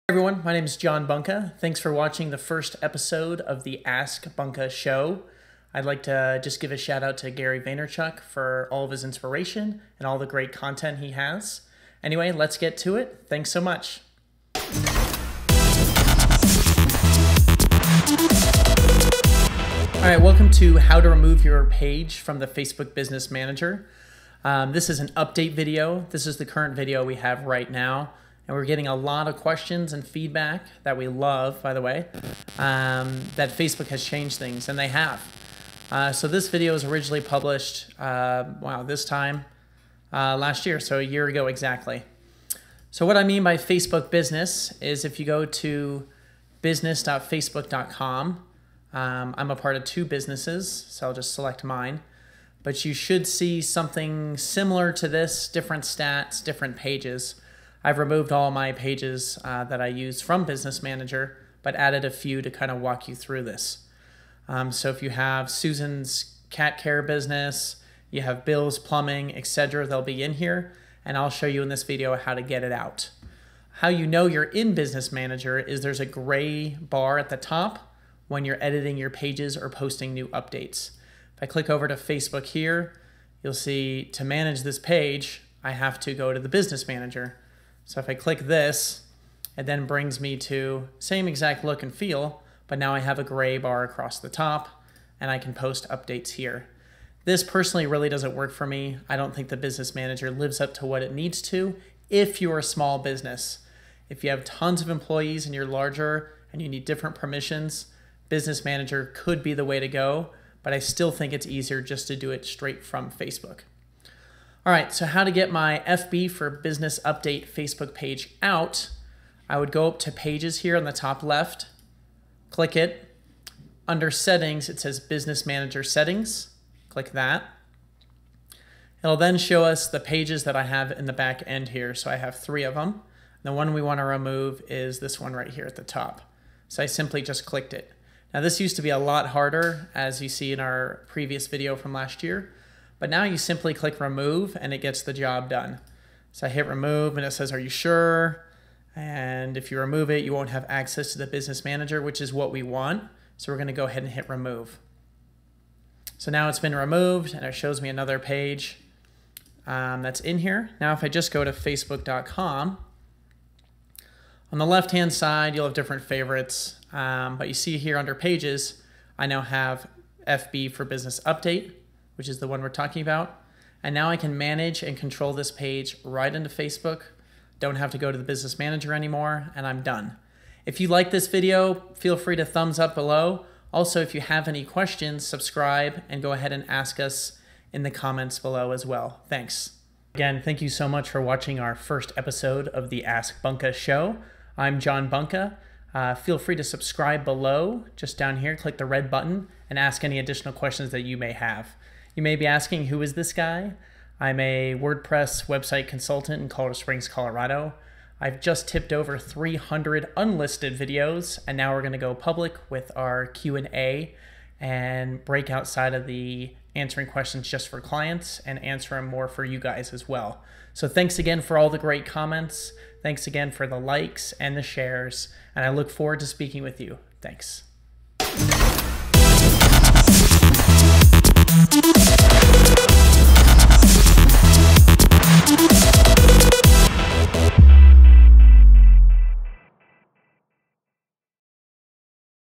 How To Remove Facebook Business Manager - AskBunka Episode 1
Did you add your Facebook Page to Facebook's Business manager but now you want out! This is the how to UPDATE!! for more business videos

Welcome to the first episode of the AskBunka Show where we will answer your questions on how to run a web business!

How To Remove Facebook Business Manager - this is an Update video as Facebook has changed how this is done which is for the better.

Instructions
To remove a Page that is connected to your business:

1. Go to Business Manager.
2. On the left side of the page, go to Business Settings and click Pages
3. Select the Page you want to remove from the left side navigation.
4. Click Remove.

This new method is fast and less confusing, so a big thanks to Facebook for updating this.

Lastly a huge shout out to the AskGaryVee show for all his amazing content and inspiration.